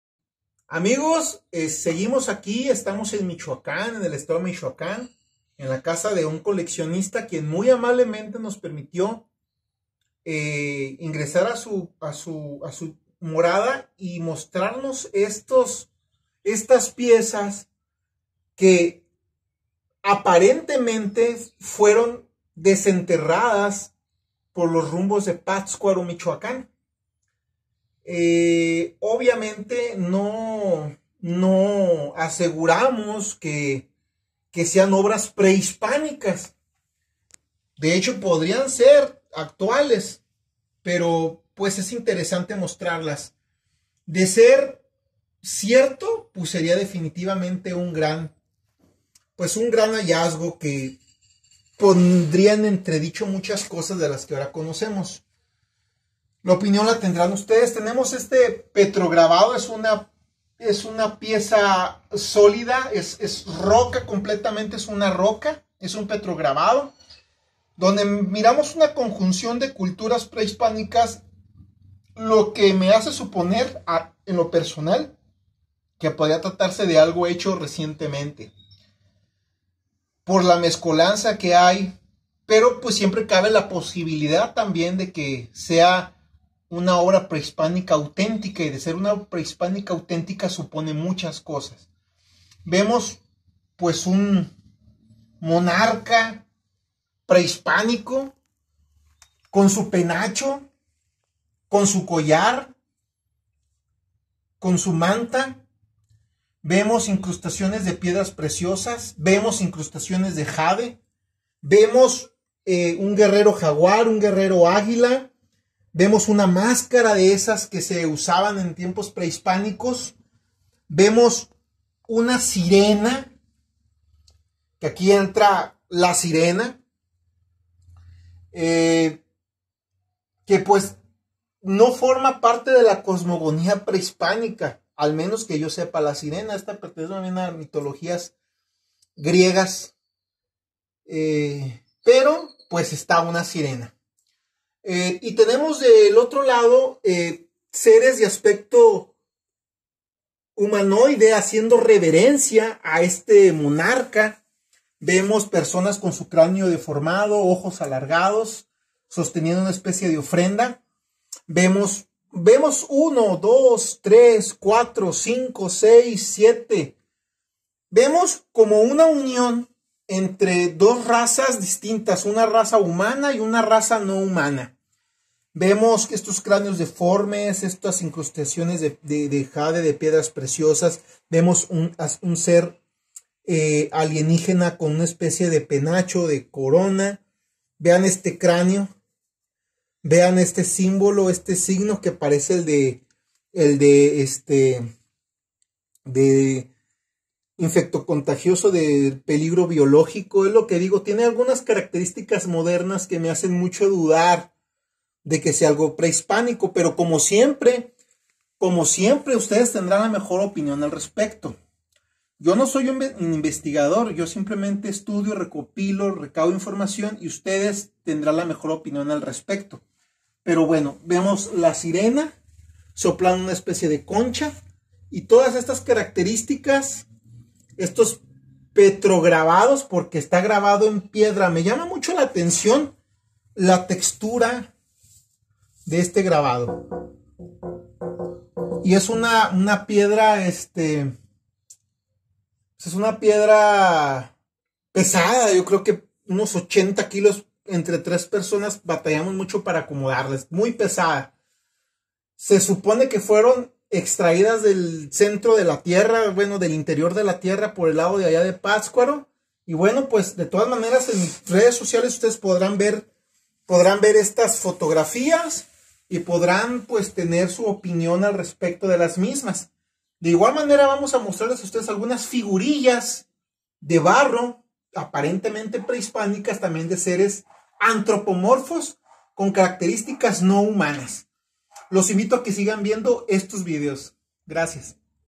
PETROGRABADOS EXTRATERRESTRES en Michoacán. Parte 2 🗿🖖🏼👽
Un coleccionista nos ha abierto las puertas de su casa para mostrarnos estos petrograbados de seres aparentemente "extraterrestres" conviviendo con antiguos purépechas.